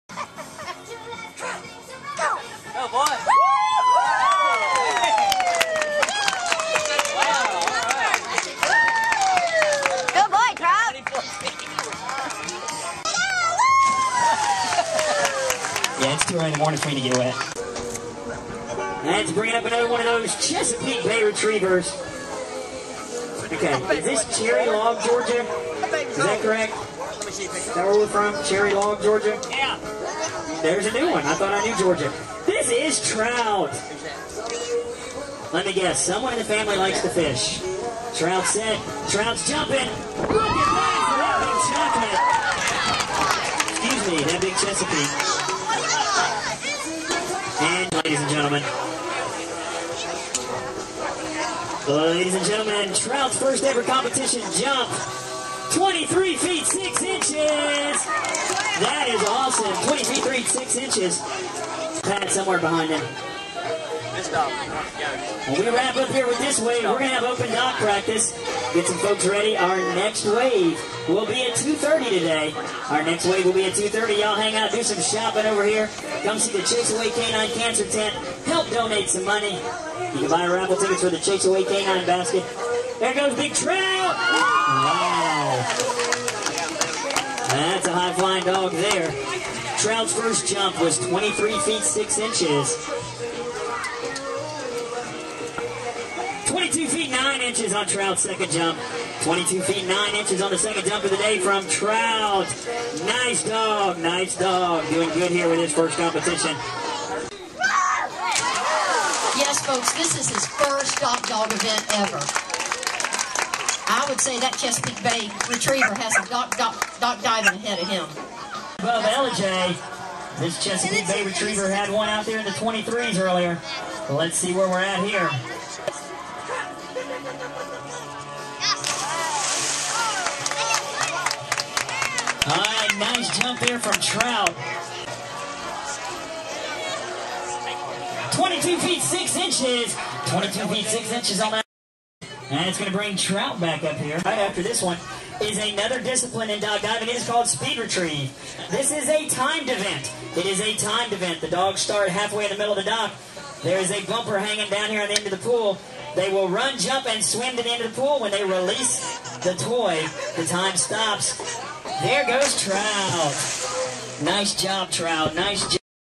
Trout Dock Diving 2010
Trout the Chepapeake Bay Retiever first Dixie Dock Dog Diving event from Athens Georgia June 2010